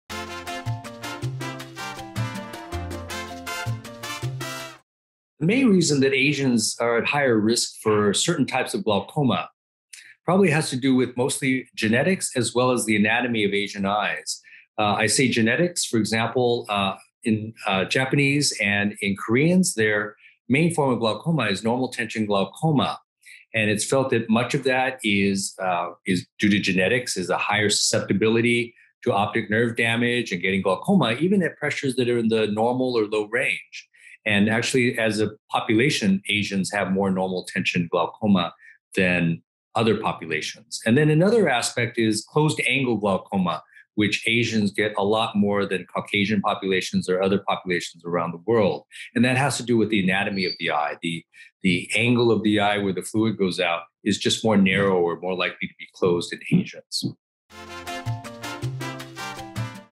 EyeFAQ: Glaucoma in Asian Populations
Question: Why do Asian populations have an increased risk for glaucoma? Answered by Shan Lin, MD, a glaucoma specialist at the Glaucoma Center of San Francisco.